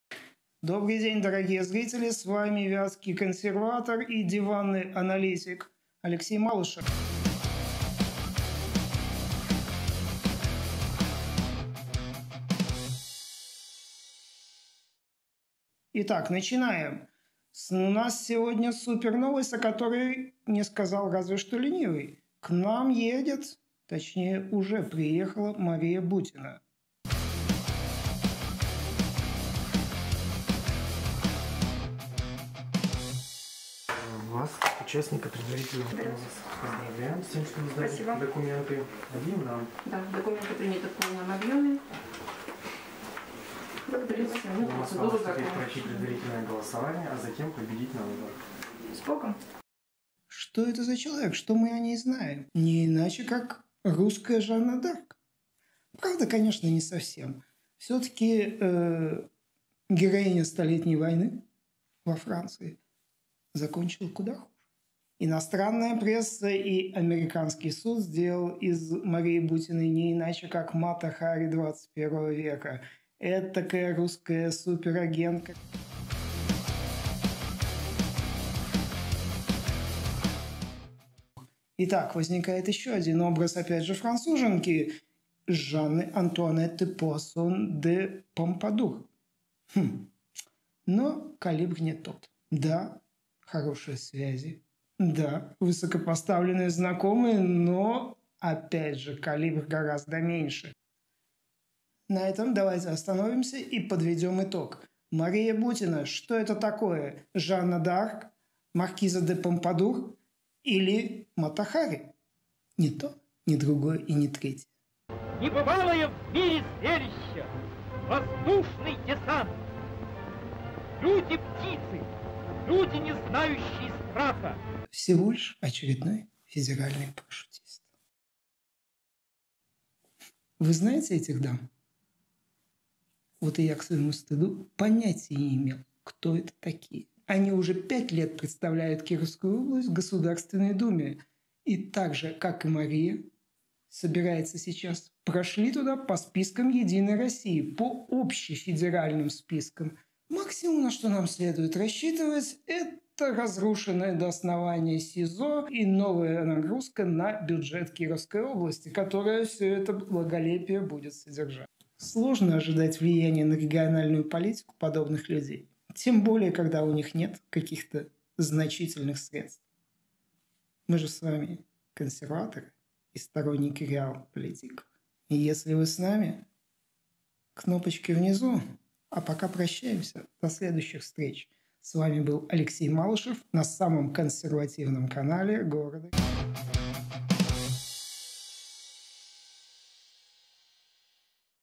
Что ждать Вятке от Марии Бутиной
Кто представляет Кировскую область в Государственной думе? О "существовании" многих депутатов не знают даже те, кто пристально следит за вятской политикой. Если гражданка Бутина войдёт в их число, изменится ли что-то для региона?